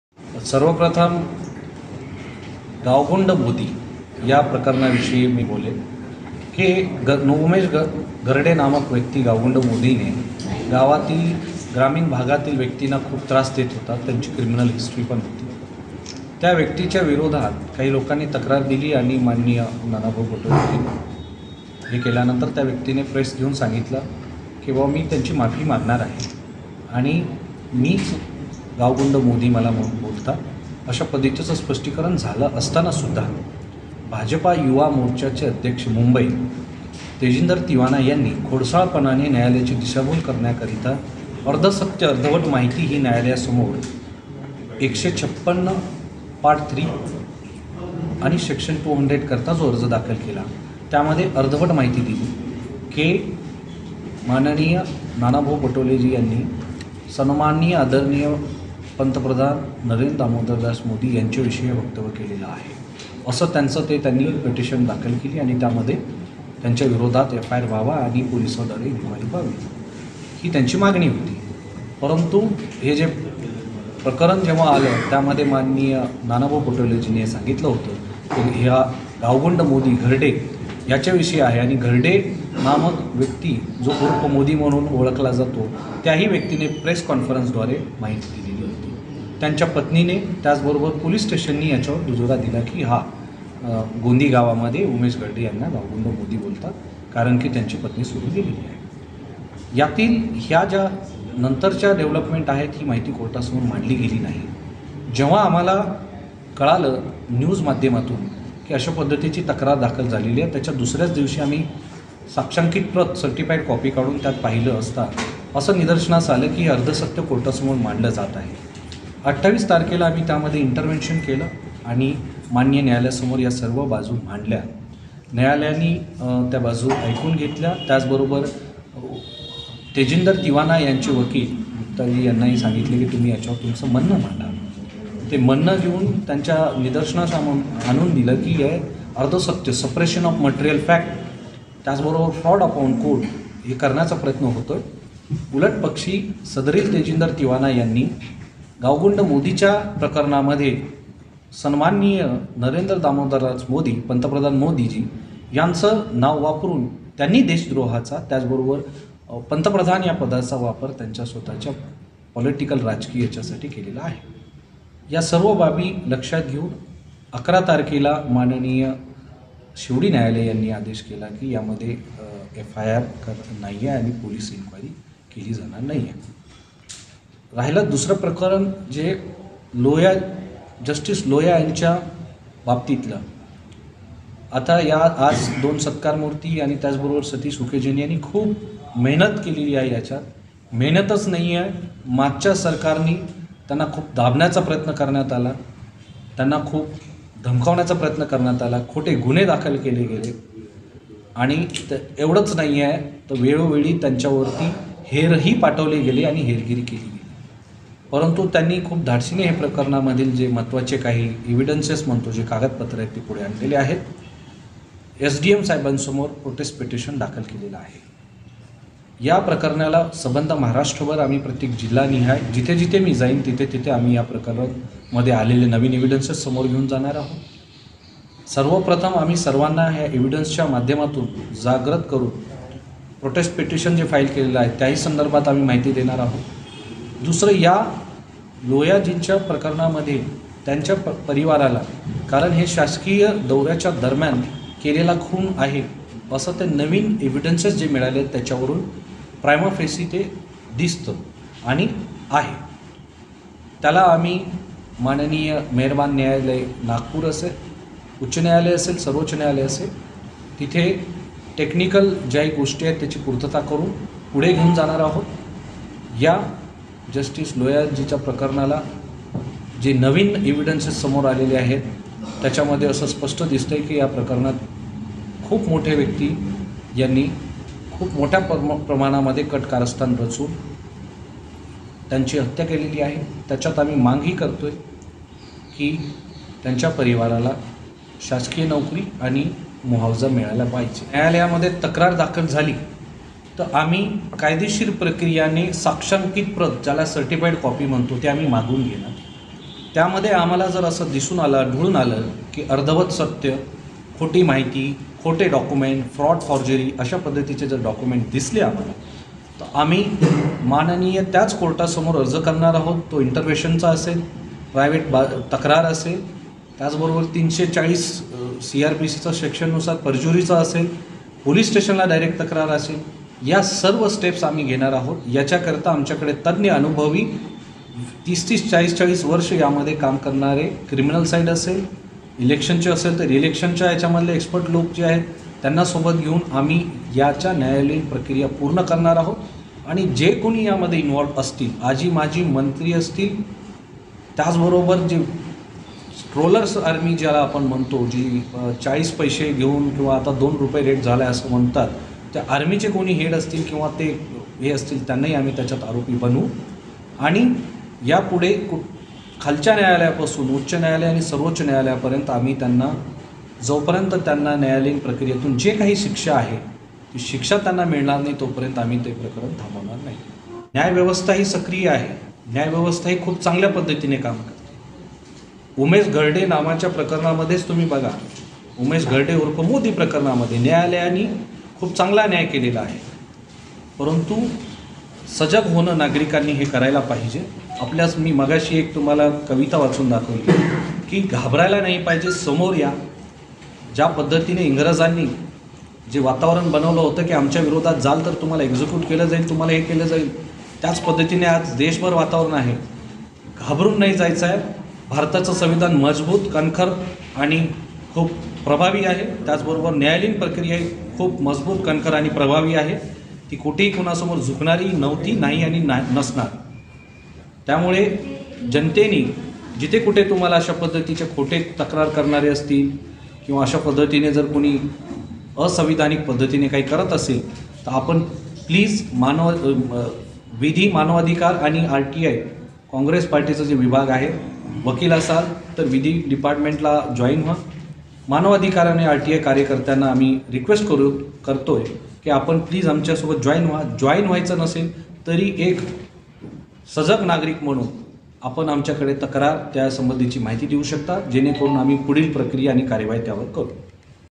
जस्टिस लोया प्रकरण, सत्य लवकरच समोर येणार, जज लोया यांच्या परिवाराला न्याय मिळणार !
MPCC (लिगल सेल) चे प्रदेश अध्यक्ष एड. रवी जाधव यांची टीम जज लोया प्रकरणाचे सत्य समोर आणणार !